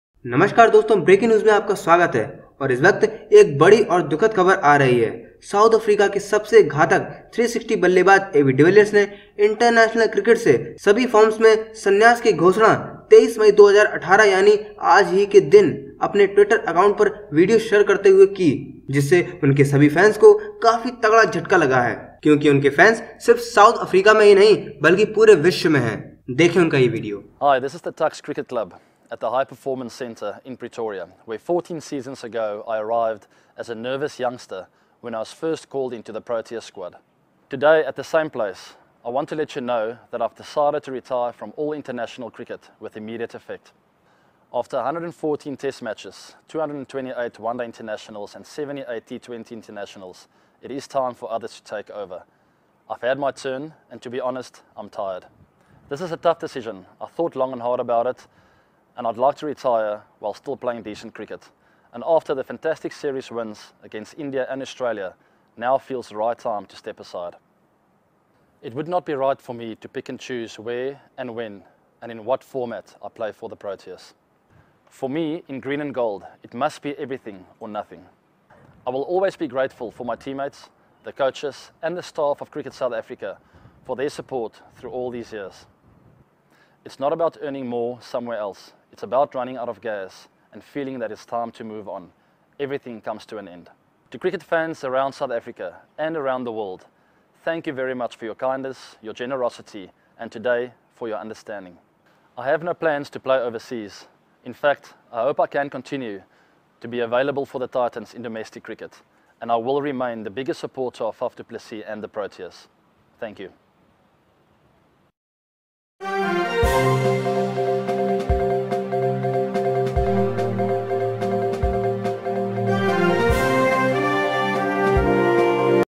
AB de Villiers ने International Cricket को कहा 'अलविदा' | AB de Villiers Announces Retirement
AB de Villiers ने International Cricket को कहा 'अलविदा' | AB de Villiers Announces Retirement from All Forms of International Cricket